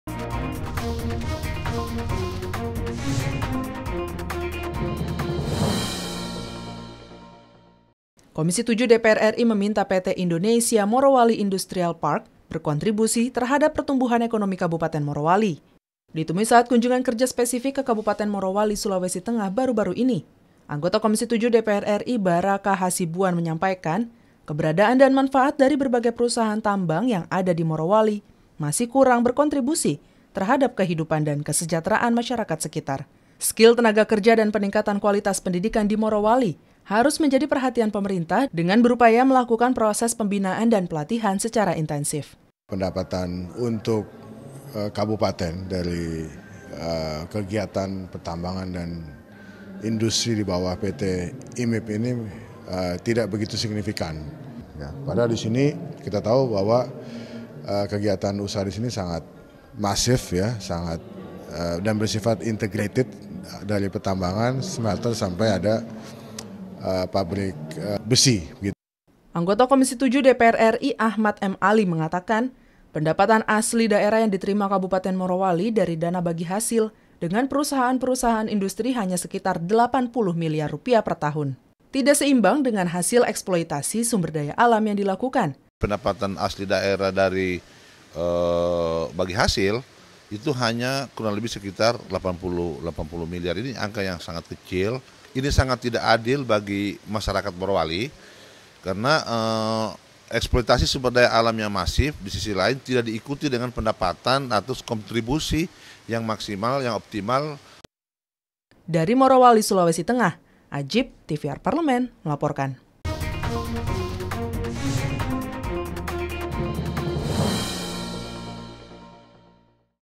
DPR RI - KOMISI VII DPR RI INGINKAN PT IMIP BANTU PAD MOROWALI
NARSUM : BARA K. HASIBUAN / Anggota Komisi VII DPR RI / FPAN / Dapil SULUT
AHMAD HI  M. ALI / Anggota Komisi VII DPR RI / FPNASDEM / Dapil SULTENG